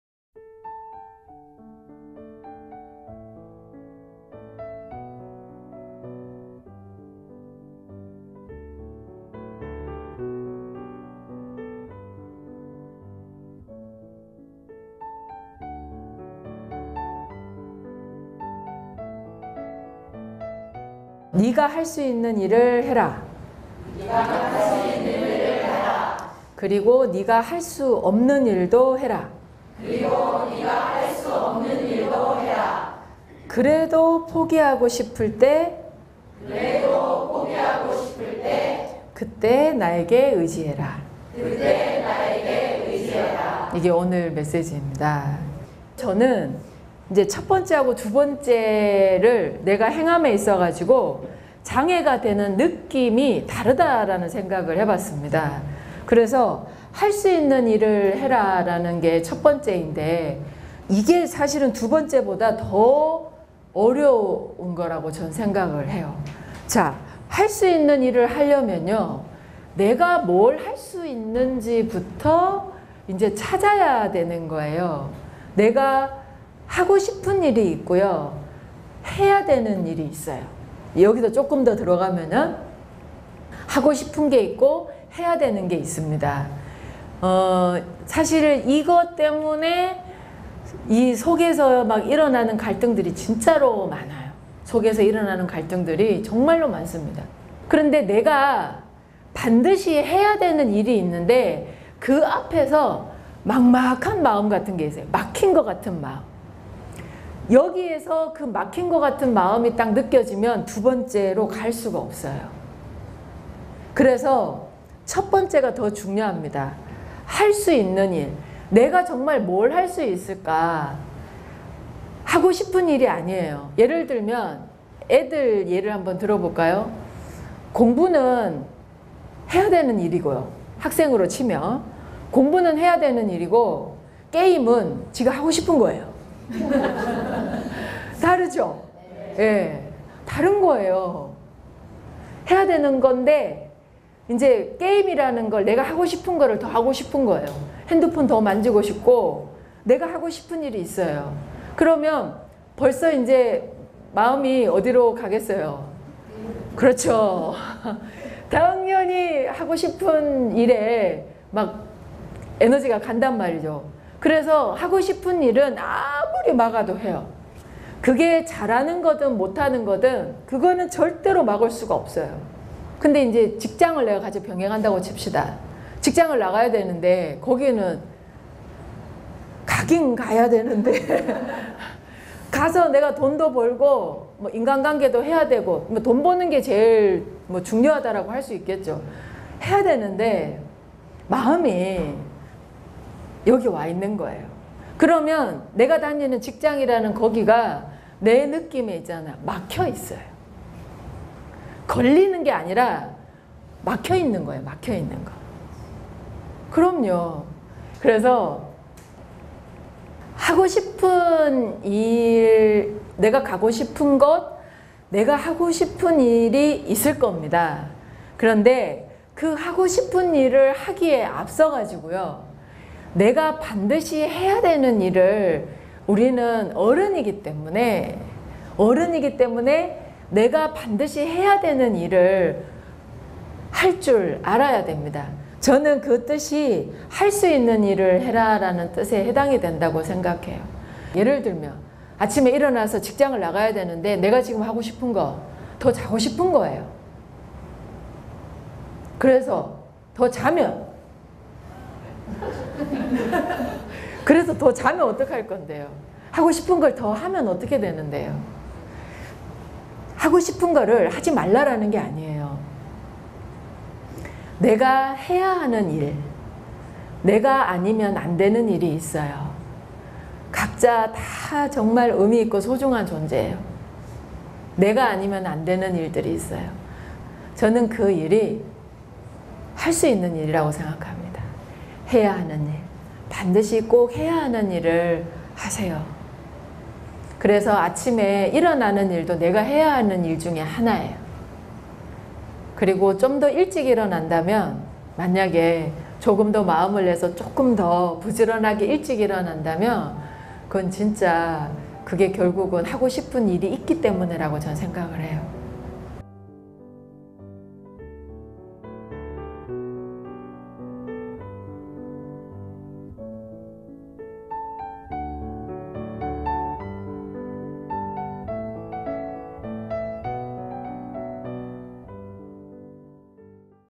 네가 할 수 있는 일을 해라 -만월도전 5분 한법 127회(영어 Manwol)
127th A Journey to Illuminating Meditation with Manwol - Do the things that you can do.

내가 해야 하는 일. 내가 아니면 안 되는 일이 있어요.
그 일이 지금 할 수 있는 일이라고 생각합니다.

Things that I have to do. The things that can be done only by myself.
I believe those are the things that I can do.